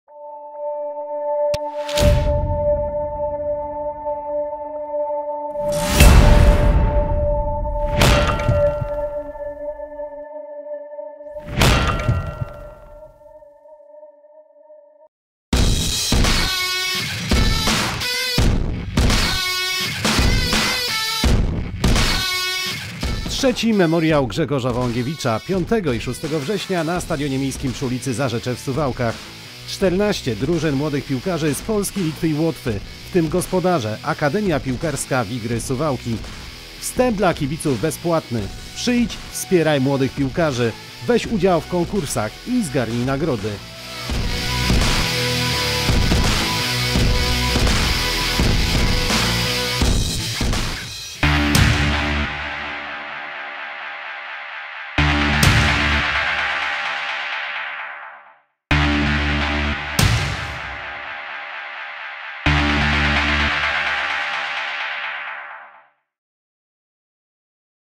III Memoriał Grzegorza Wołągiewicza. 5,6 września!
W najbliższy weekend odbędzie się III Memoriał Grzegorza Wołągiewicza, byłego prezydenta Suwałk i prezesa SKS Wigry.
Na płycie głównej stadionu miejskiego przy ulicy Zarzecze w Suwałkach zaprezentuje się 14 zespołów dziewięciolatków i młodszych z Litwy, Łotwy i Polski. Zapowiadają się emocje nie mniejsze niż podczas spotkań pierwszoligowych Wigier. Na kibiców czekają też konkursy z cennymi nagrodami. Wstęp na mecze jest bezpłatny.